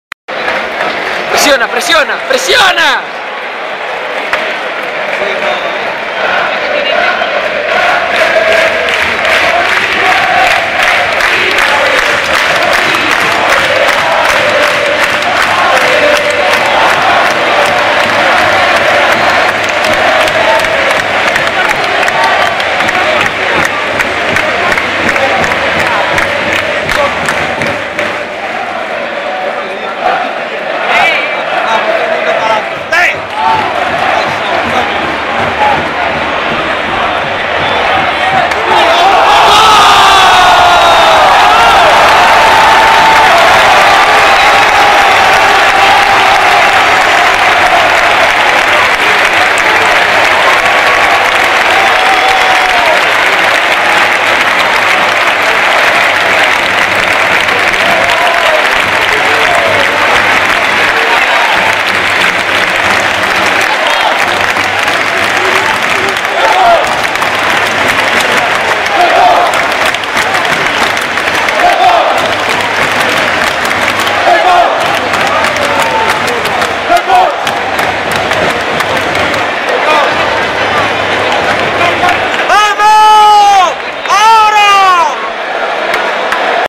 Deportivo-Osasuna: Gol de Riki, 1-0 (20/08/2012)
Gol de Riki en una bonita jugada individual de disparo raso ajustado al poste, 1-0. Resultado final 2-0!